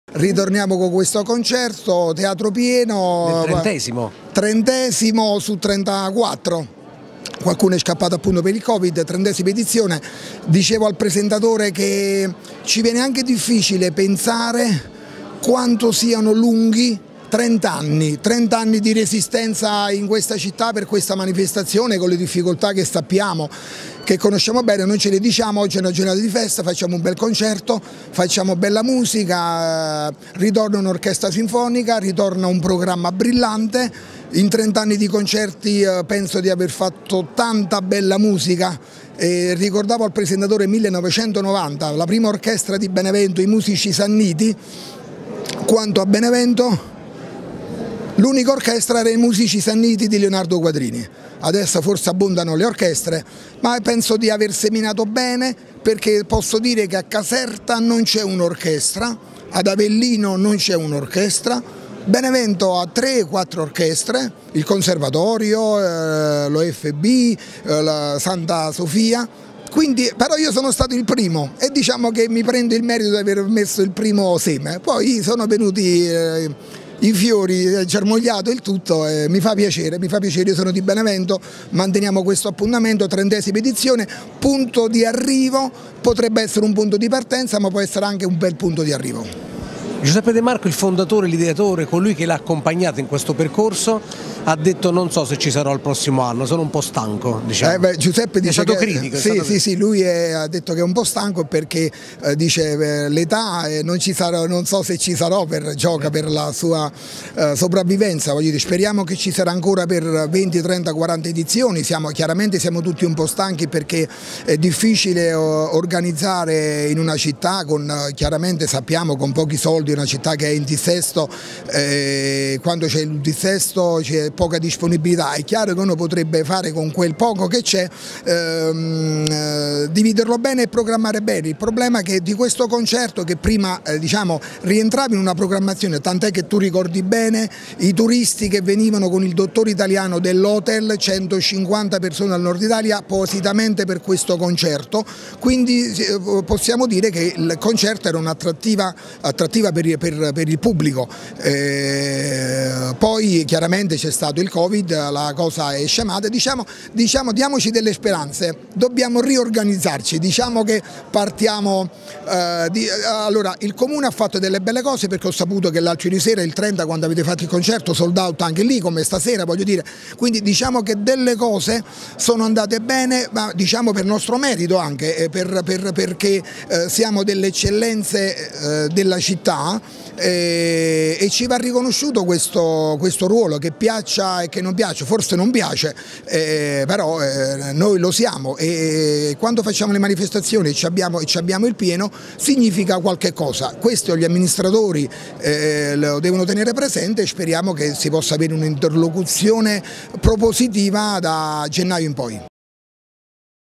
SUCCESSO PER IL TRENTENNALE DEL CONCERTO DI CAPODANNO SUL PALCO DEL TEATRO COMUNALE
L'arrivo del 2023 è stato salutato dalla XXX edizione del  GRAN CONCERTO DI CAPODANNO.
Dopo due anni di  stop dovuti al rispetto delle norme anticovid, ad ospitare lo storico evento artistico ideato da Peppe De Marco, è stato il rinnovato Teatro Comunale Vittorio Emmanuele di Benevento.
Sul palco l’Orchestra Internazionale della Campania diretta dal Maestro Leonardo Quadrini con la partecipazione straordinaria del M° Nello Salza (la tromba storica di Ennio Morricone) che hanno dato  il benvenuto al 2023 tra musica ed emozioni.

TV7 ha seguito lo straordinario concerto che ha fatto registrare il pienone delle grandi occasioni.
Sentiamo l'intervista al Maestro Leonardo Quadrini realizzata da Alfredo Salzano